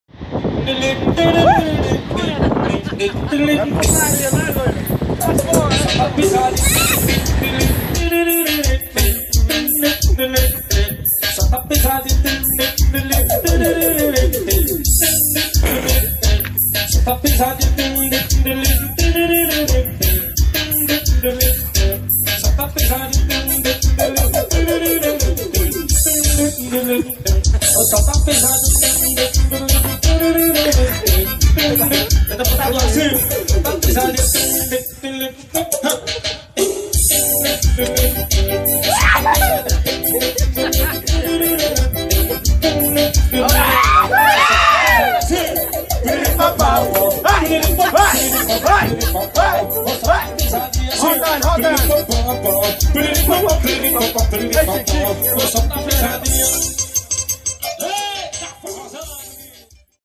Pisadinha do fim de semana
Momentos do nosso passeio no litoral,
Muito legal, ótimos momentos em família.